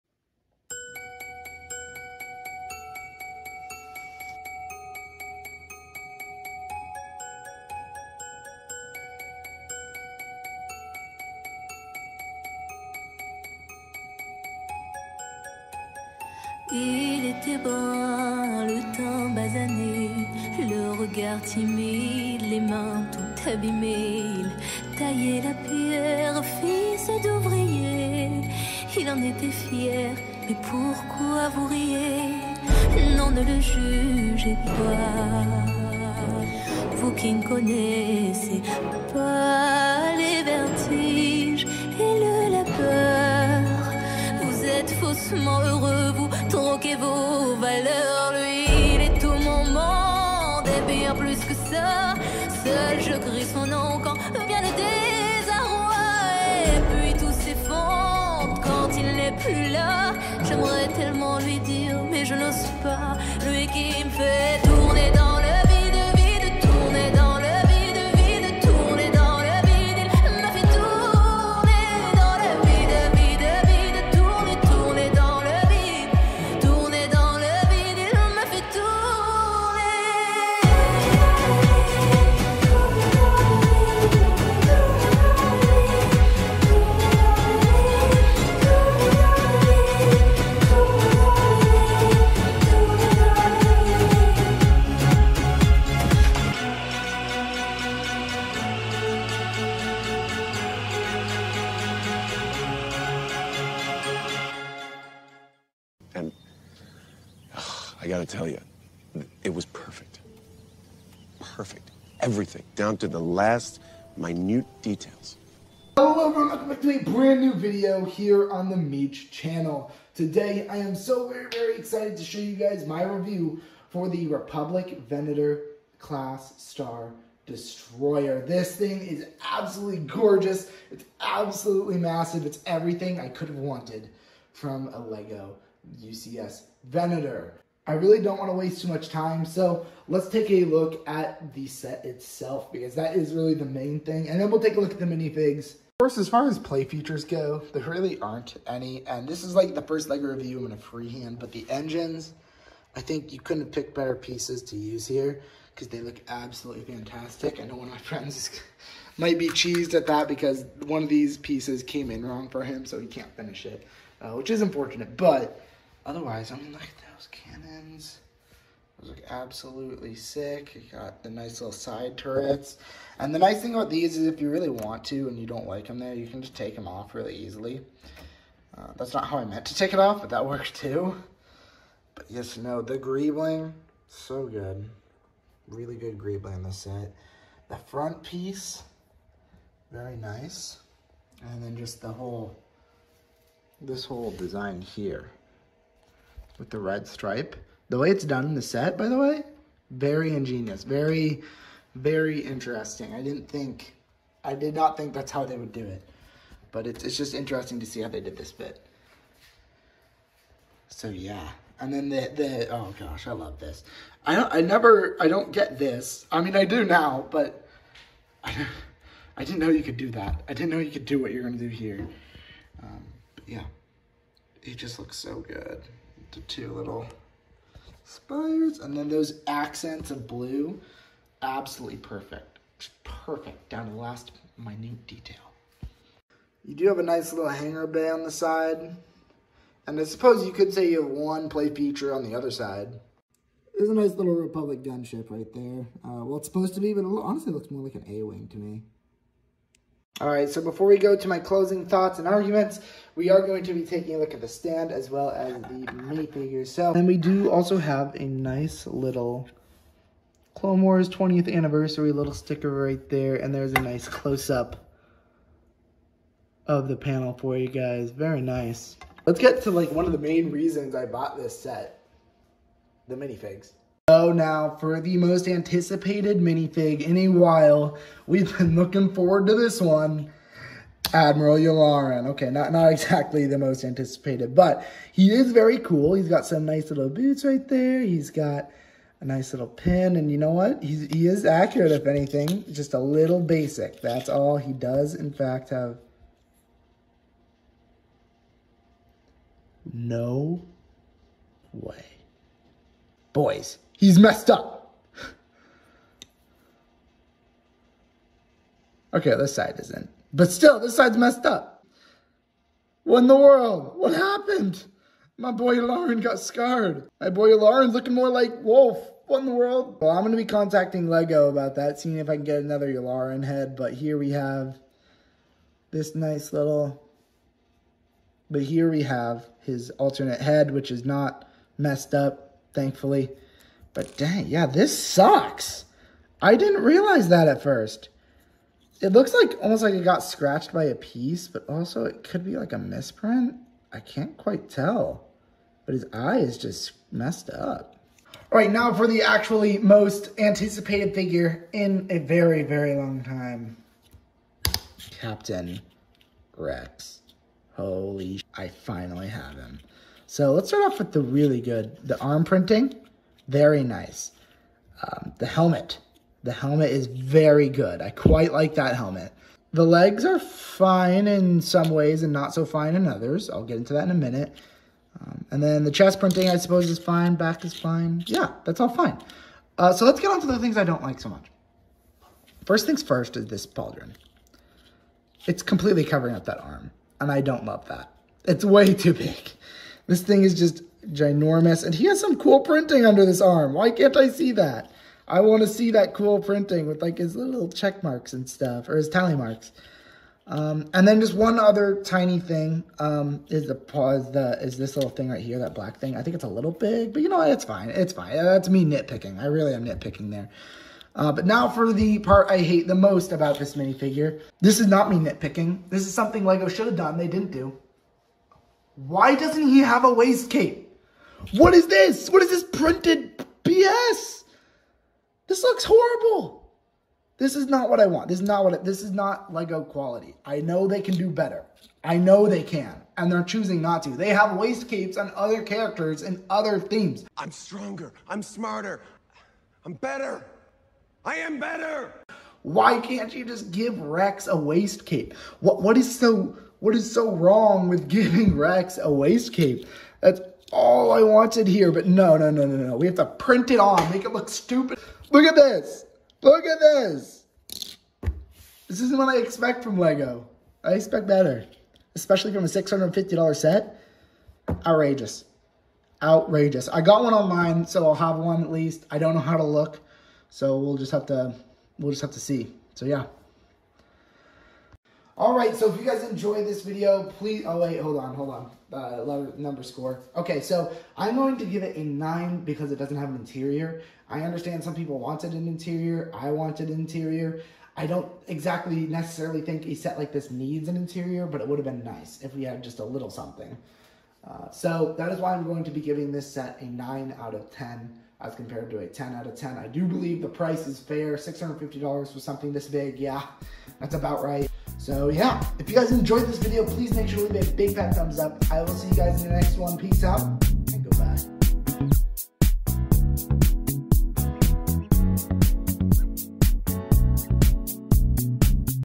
Lego UCS Venator-class Star Destroyer!
In this video we review the Lego UCS Republic Venator-class Star Destroyer. This set is $650 and includes the highly anticipated Captain Rex and Admiral Yularen Minifigures.

0:00 Meach Tate
1:45 Intro
2:25 Review
4:09 Stand
4:33 Admiral Yularen
6:05 Captain Rex
7:10 Meach Rants About Waist Capes
8:15 Outro